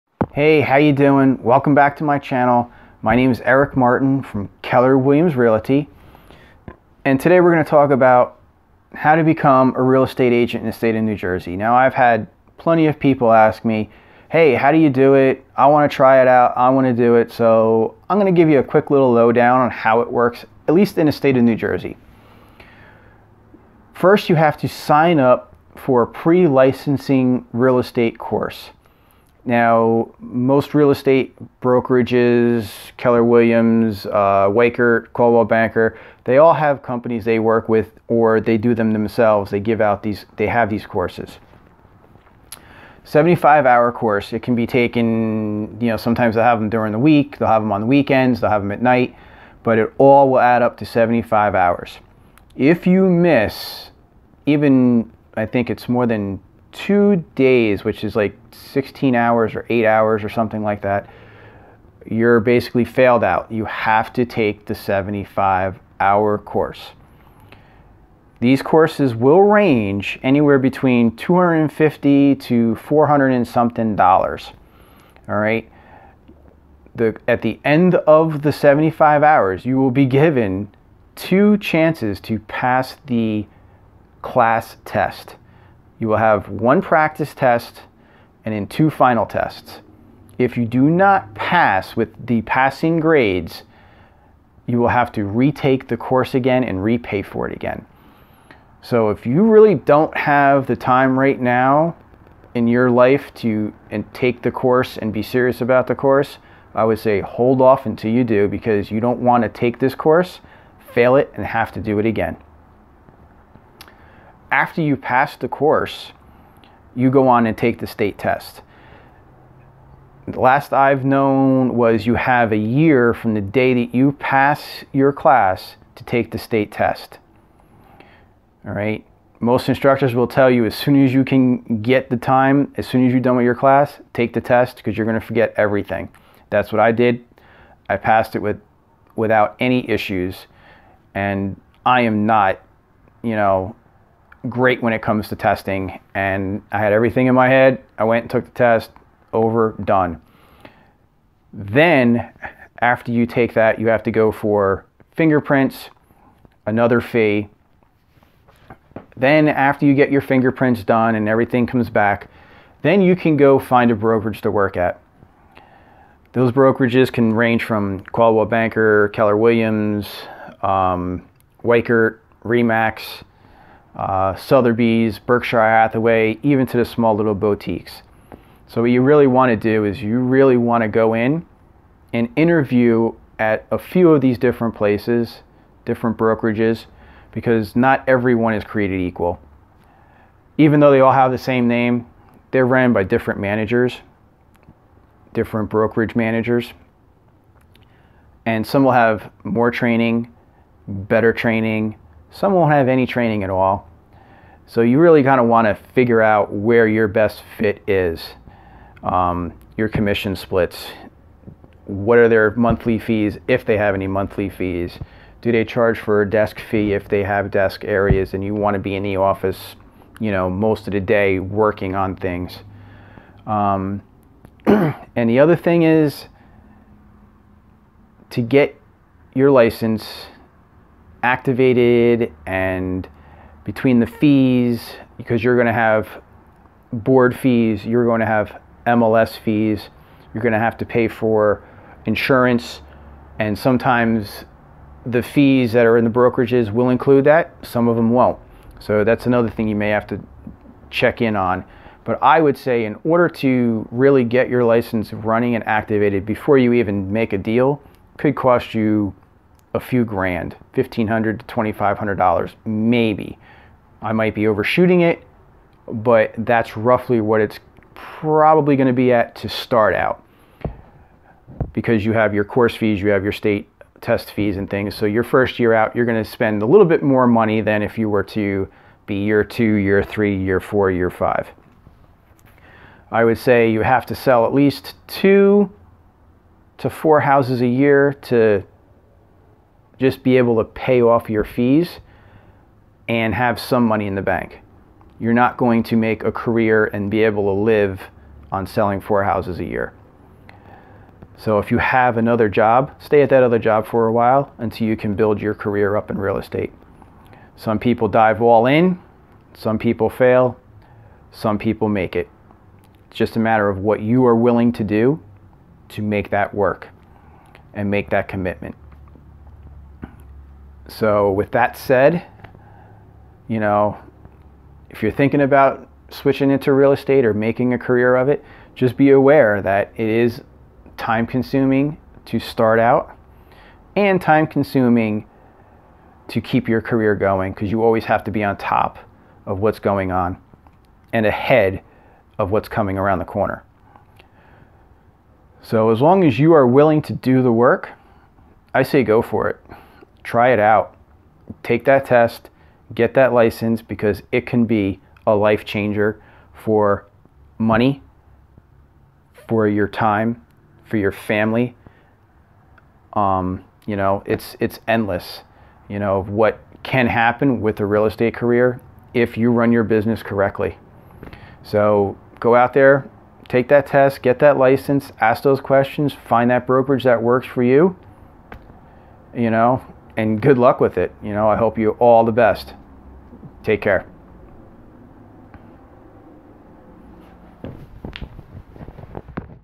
How to become a realtor.
In this video I tell you how to attain a N.J real estate license and how to become a realtor. I explain the class process, state test and associated costs with getting your real estate license.
Facebook: Eric Martin Keller Williams Metropolitan